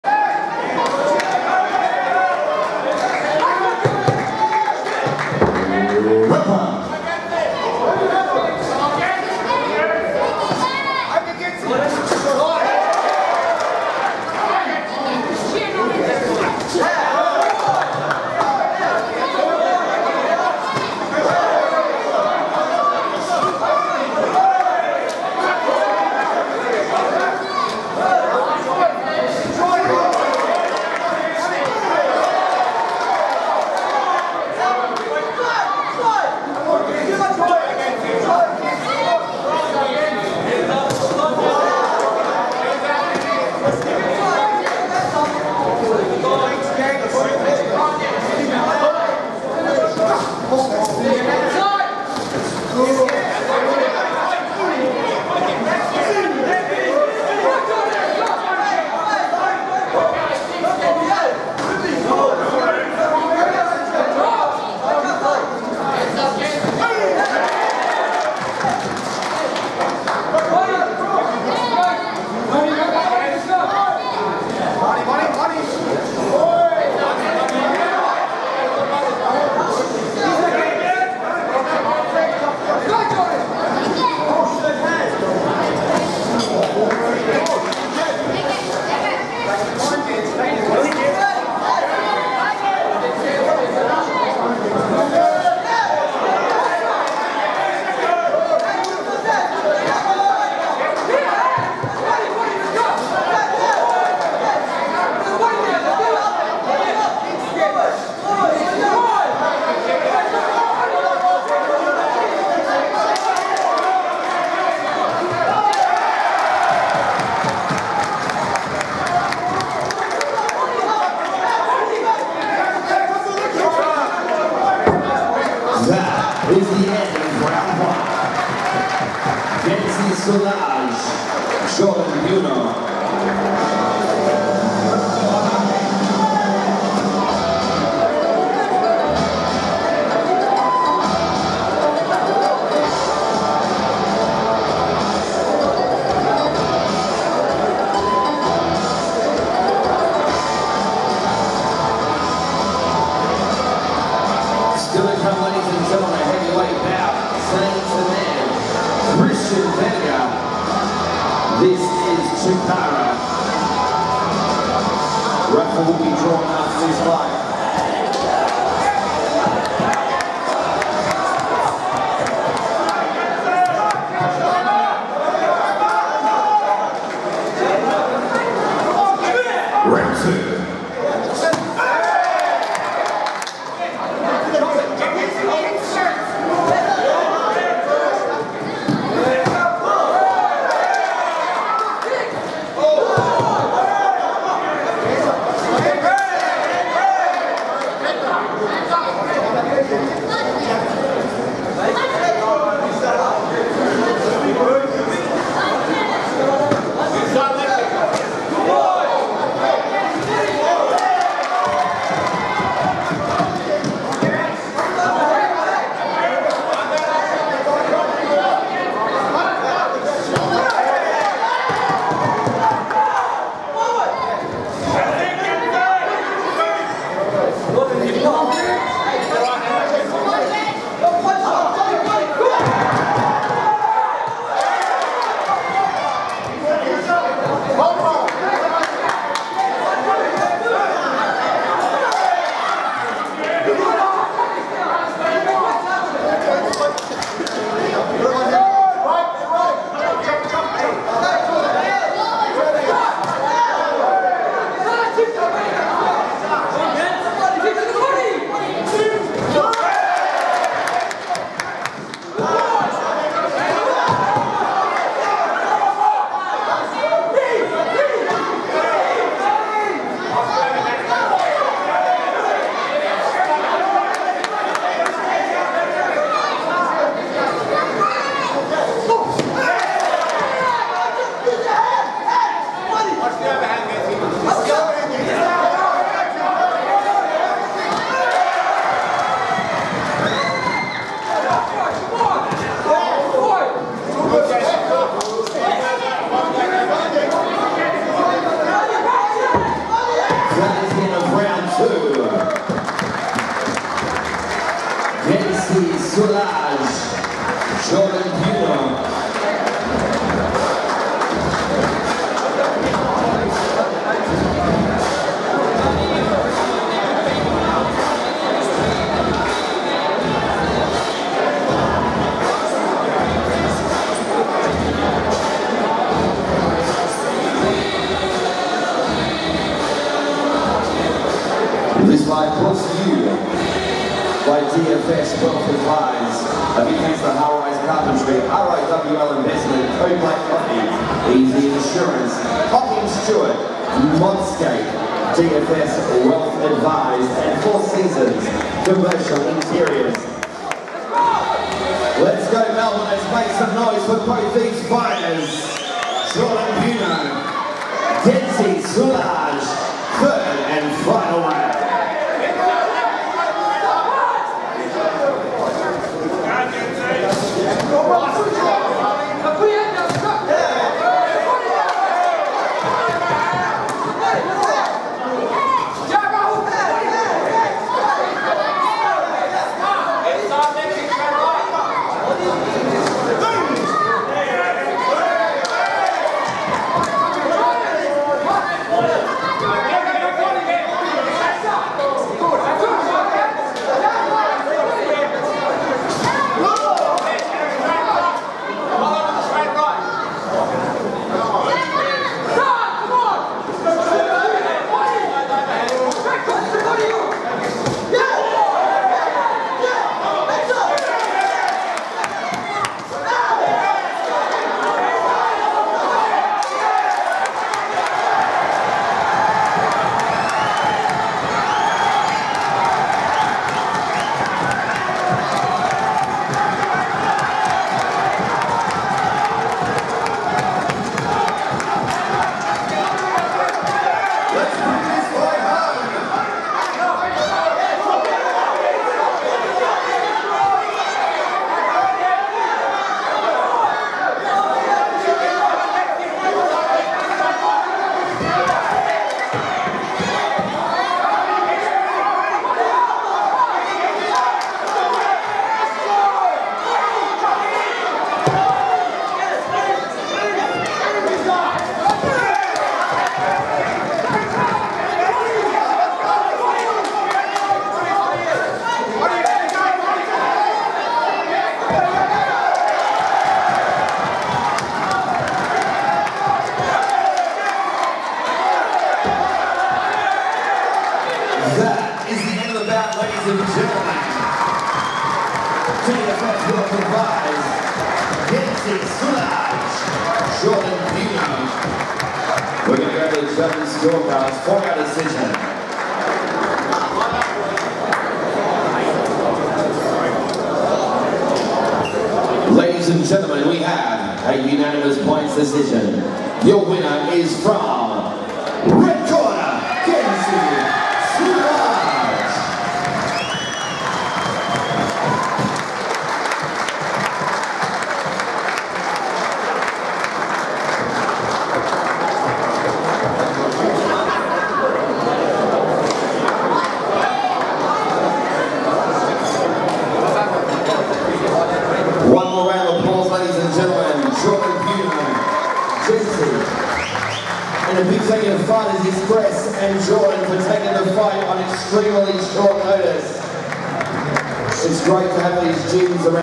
CHIKARA - Full Contact Karate with Head Punches. Genci Sulaj vs Jordan Buorno
Chikara Fight Night. 3 x 2 minute rounds. Full Contact Karate.
March 26th 2022. Melbourne. Australia.
Full Contact Karate with Head Punches.
15 year old Genci demostrated that his hard work and hard training pays off.